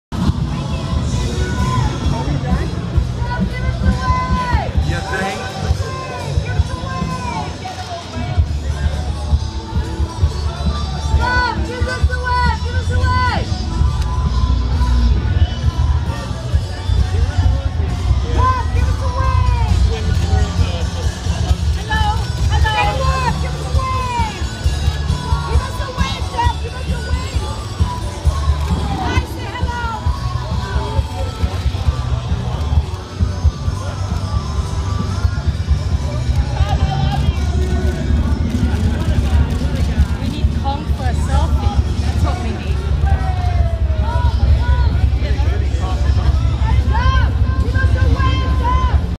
Tom Hiddleston at the Premiere of Warner Bros. Pictures' "Kong: Skull Island"
Tom Hiddleston at the Premiere of Warner Bros. Pictures' "Kong: Skull Island" held at the Dolby Theatre in Hollywood, CA, March 8, 2017. Credit: Joseph Martinez / PictureLux